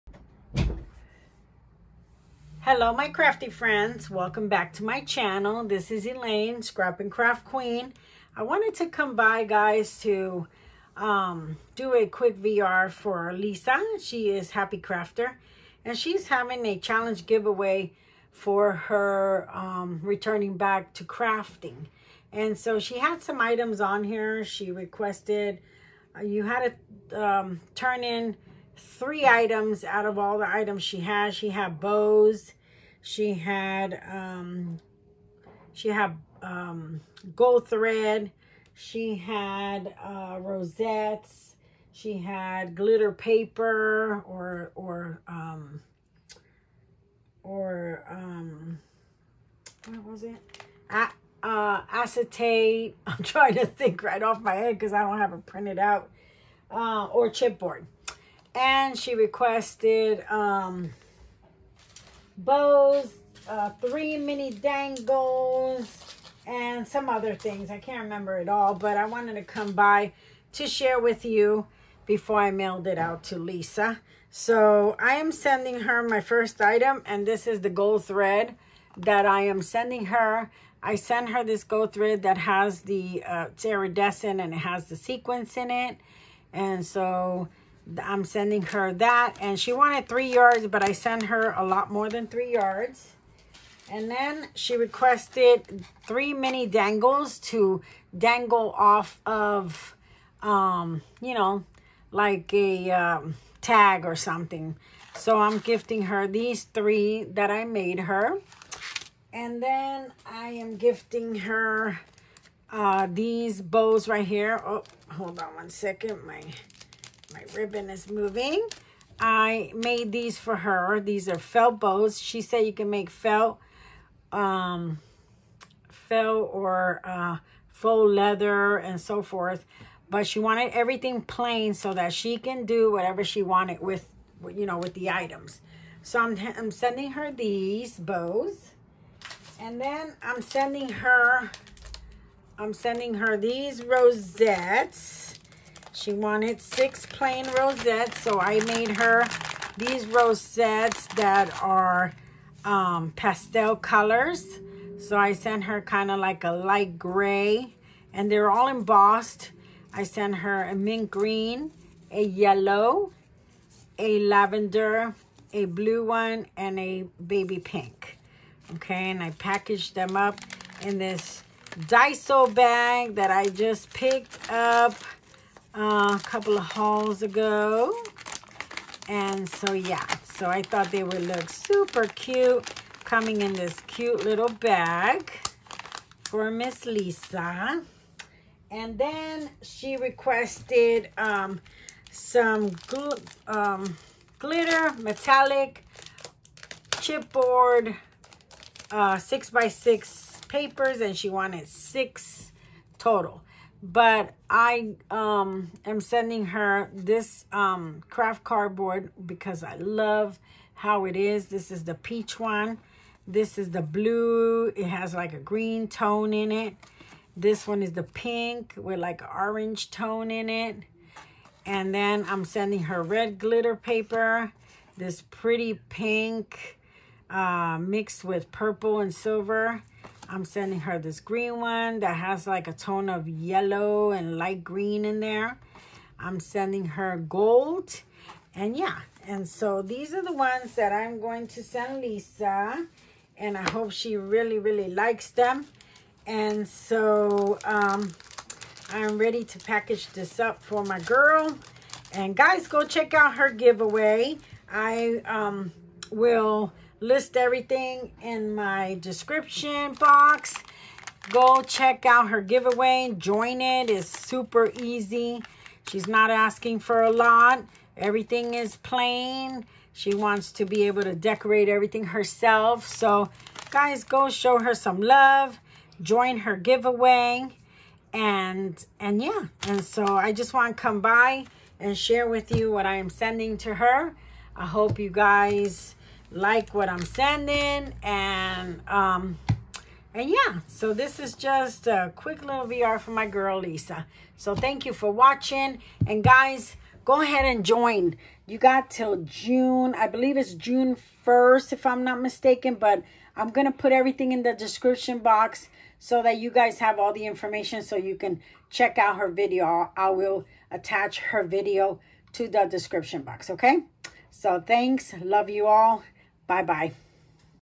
VR For @happycrafter Challenge Giveaway
Hello Crafty friends,
I wanted to come by to share this VR for Lisa Goss she is Happycrafter on YouTube. She just hit a 3000 subbies and is returning back to crafting. I linked the video to her giveaway go check it out and join.

The contents of my videos are not intended for Children and are for Adults Only.